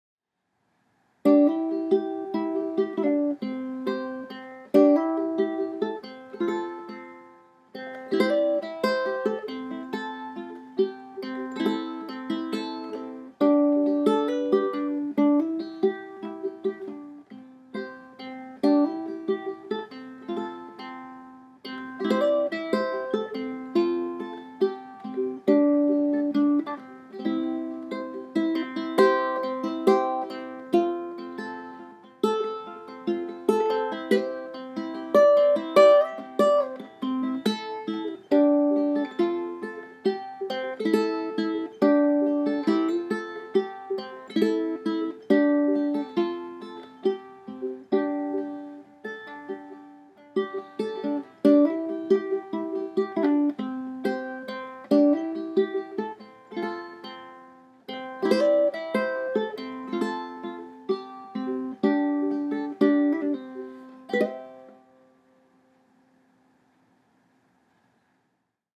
Monday Morning Uke
Here's something nice and gentle to ease into Monday with.  Noodling around on a little bit of Ukulele with my morning coffee and pyjamas.